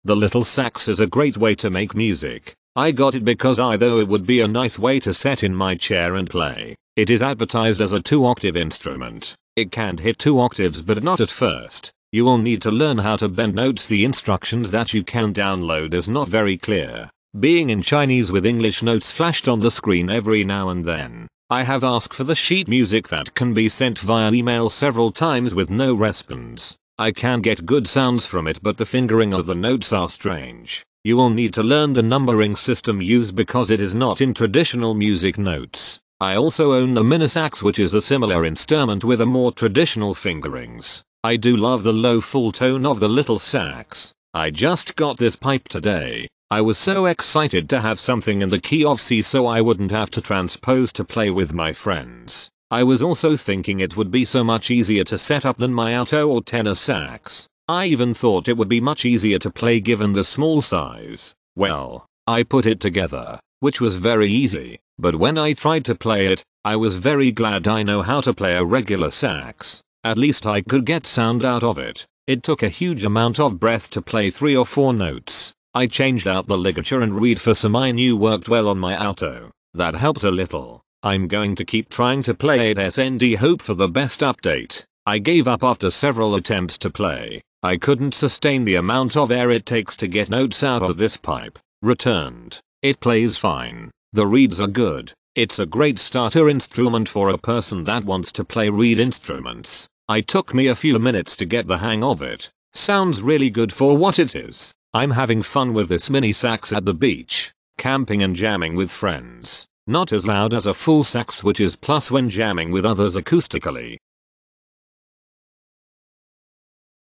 HiXing Simple sax,Mini Saxophone, Pocket Sax for C Key ,Adult students and beginner review
HiXing Simple sax,Mini Saxophone, Pocket Sax for C Key ,Adult students and beginner professional performance

Main Features:
- Use alto saxophone mouthpiece, blow same way as saxophone
- Range is 2 octaves, C key, main blow F key, with simple fingering
- its timbre like a saxophone
- Easy to carry and play anytime and anywhere
- There are special teachers, you can consult us at any time.